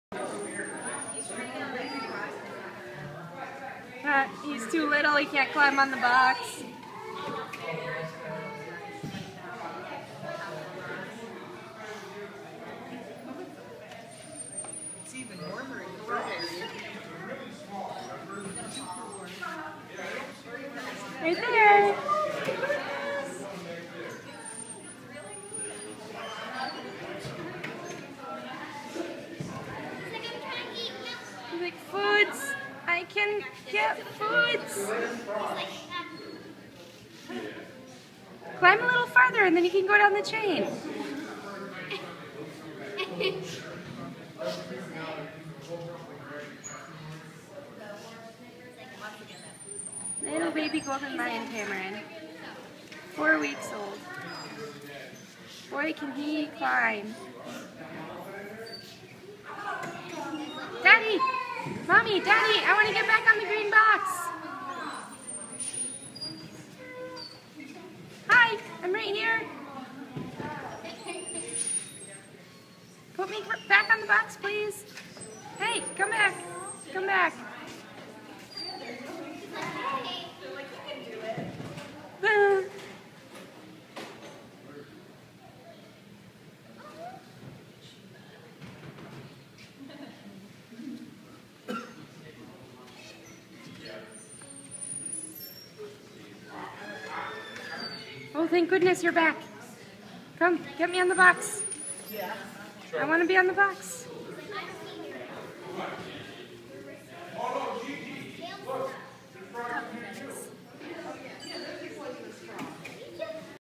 Adorable baby Golden Lion Tamarin!
4wk old Golden Lion Tamarin at the Seneca Park Zoo